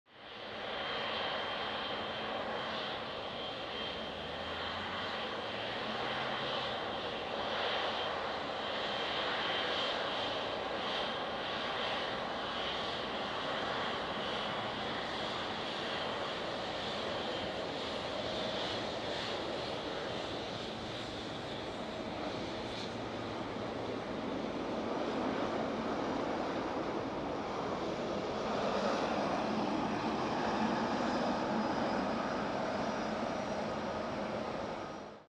Air Canada Boeing 777 at YYZ
Air Canada Boeing 777-333(ER) off to India